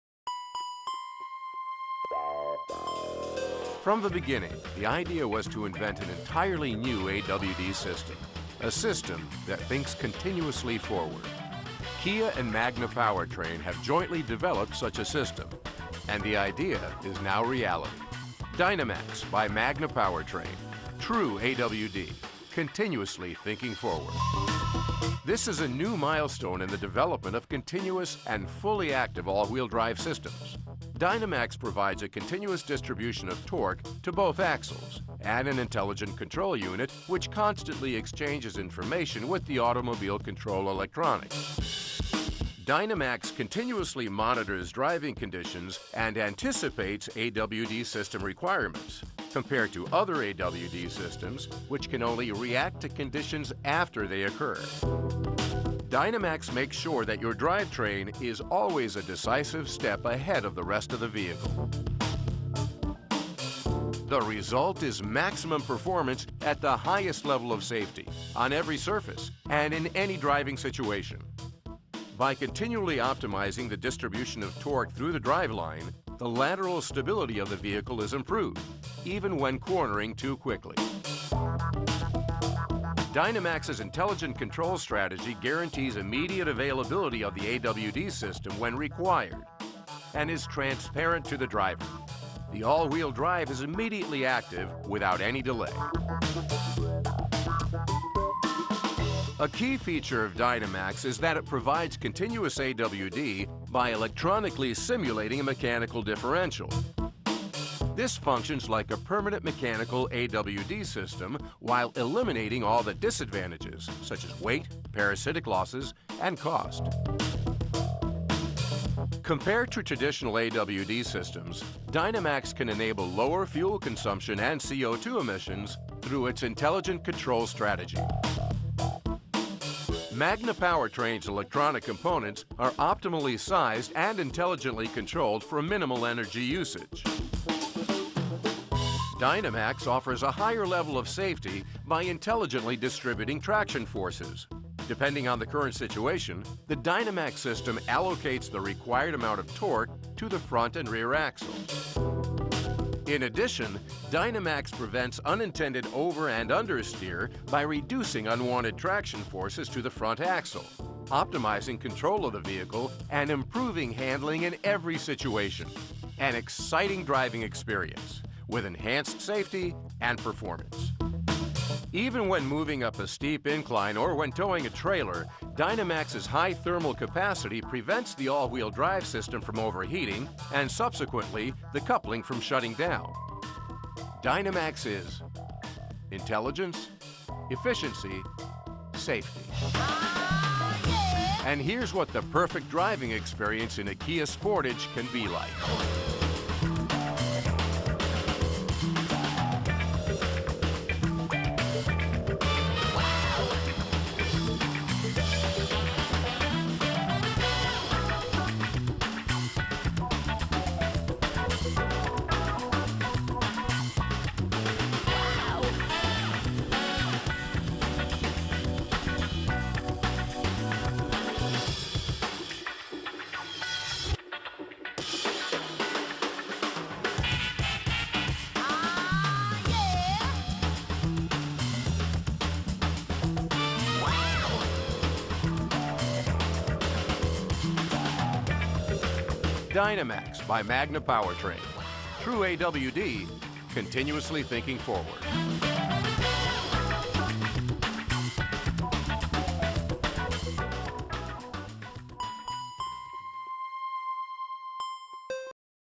Kia Sportage AWD System
Kia Motors and Magna Powertrain, part of Magna International Inc, announced the official launch of the new Dynamax™ All-Wheel-Drive (AWD) system in the all-new Kia Sportage. Dynamax continuously monitors driving conditions and anticipates AWD system requirements, compared to other AWD systems which can only react to conditions after they occur.